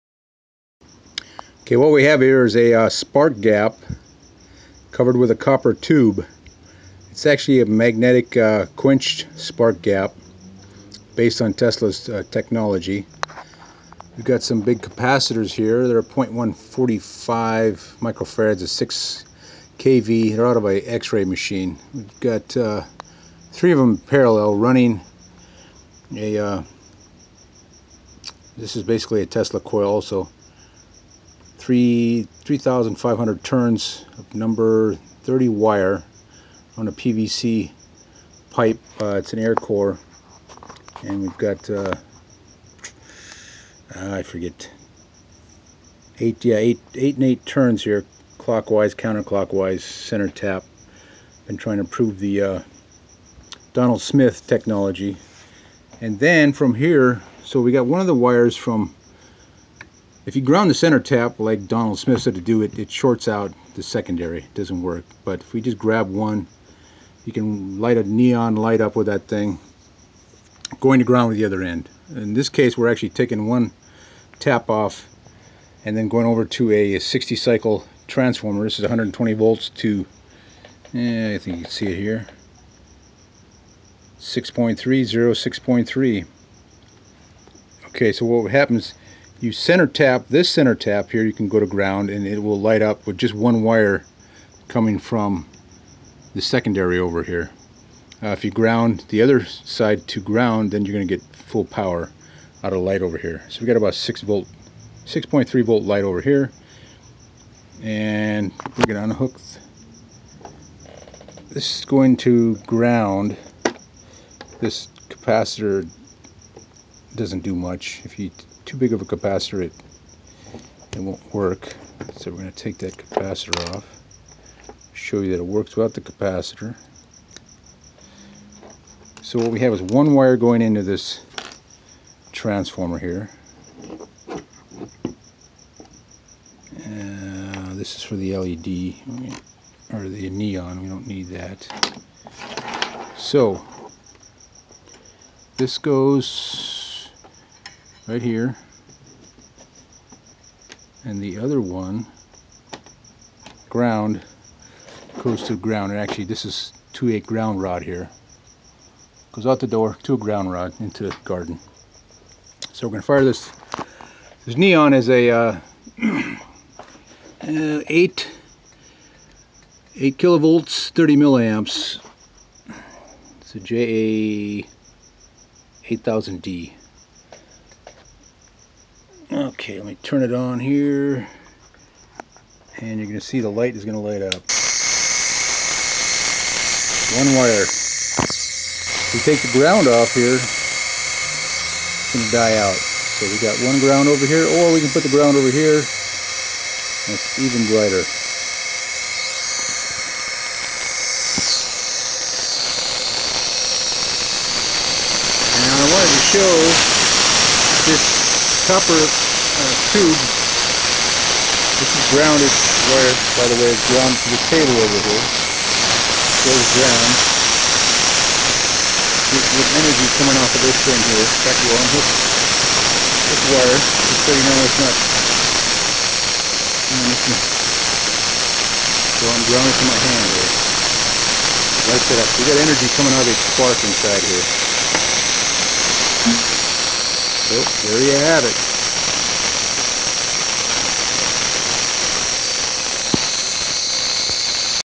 Donald L. Smith exp. 21 (Cold electricity)
This circuit is testing various aspects of the Donald. L. Smith circuit as well as some aspects of Ed Gray's.

After running the spark gap at full power and .050 gap for over an hour, the electrodes were both cool to the touch. the arc was very bright and noisy as can be heard in the video.

The spark gap uses 6 tungsten welding rods for 1 electrode and a threaded bronze rod for the other. Tesla used bronze rods with large balls on the ends. Bronze seems to work better than stainless steel. The welding rods are bundled up as one electrode. This allows better heat dissipation and is a lot cheaper than buying one 1/4 inch tungsten rod instead.

The neo magnets are Diametrically Magnetized and have a few layers of Mica to keep the arc from jumping on the magnet. the distance between the electodes and the magnets is adjustable. The holder for the whole spark gap assembly is made from phenolic sheet stock 1/4 inch thick.

The magnets are 5/8 inch diameter and 5/8 inches tall. buy them

 Welding Tungsten Electrodes 2% Thoriated (Red) 1/16"

Phenolic sheet

The Free energy secrets of cold electricity (Ed Gray, Tesla's work)